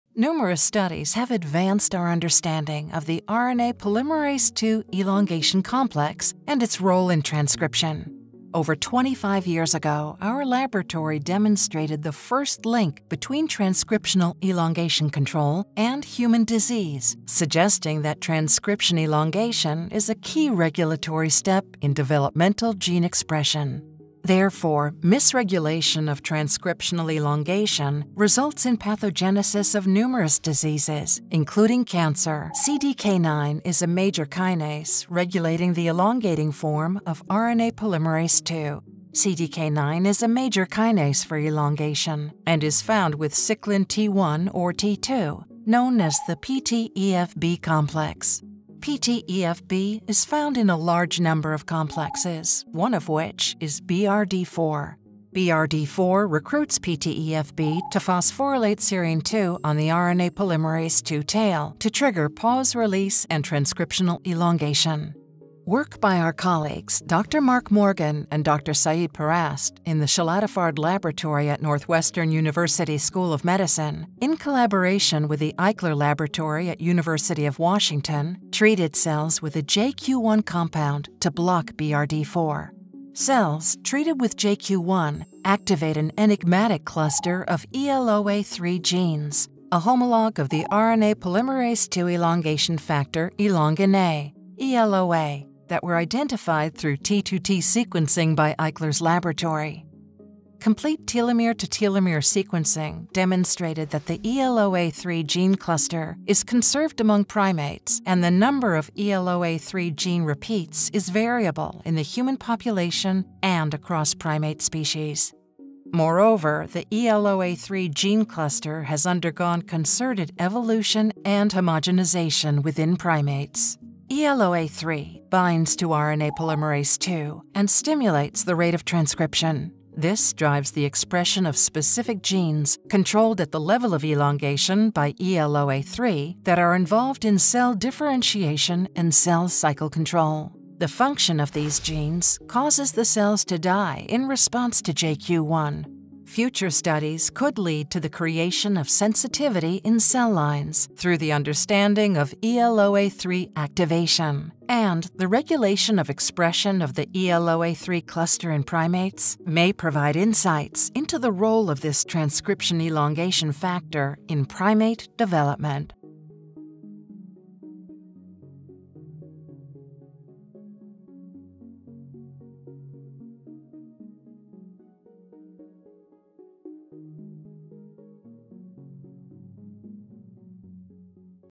ELOA3: Primate-specific RNA polymerase II elongation factor encoded by a tandem repeat gene cluster
A recent publication in Science Advances from the Simpson Querrey Institute for Epigenetics in the Shilatifard Laboratory demonstrates the ELOA3 gene cluster is conserved among primates and the number of ELOA3 gene repeats is variable in the human population and across primate species. Moreover, the ELOA3 gene cluster has undergone concerted evolution and homogenization within primates. Future studies could lead to the creation of sensitivity in cell lines through the understanding of ELOA3 activation.